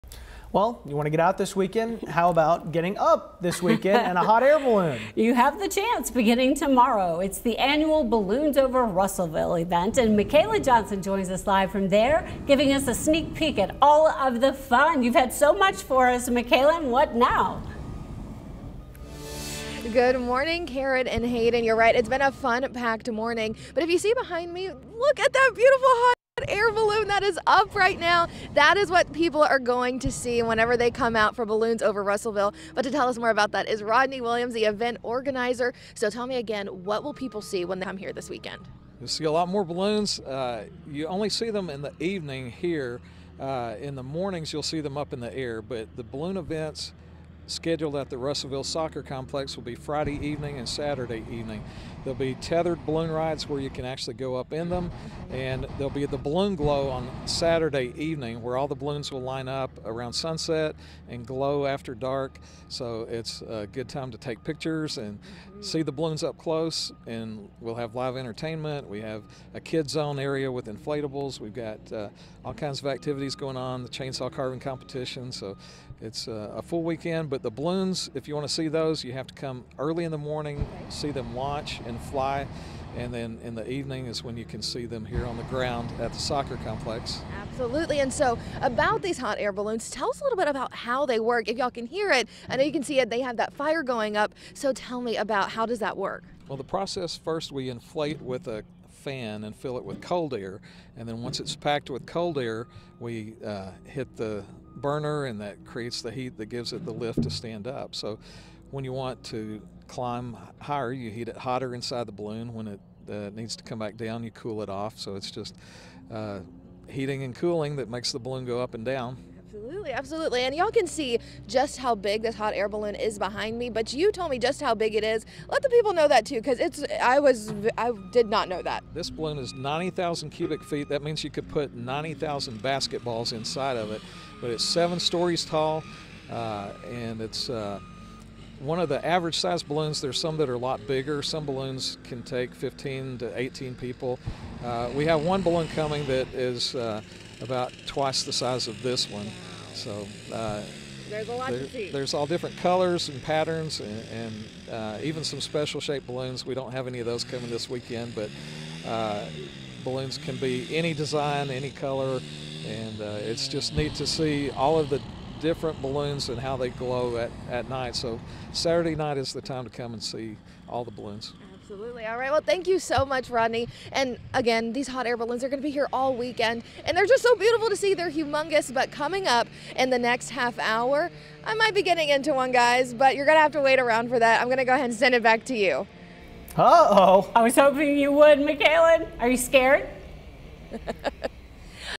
Sneak peek at Balloons over Russellville
Want to get out this weekend? Well, how about getting up— in a hot air balloon!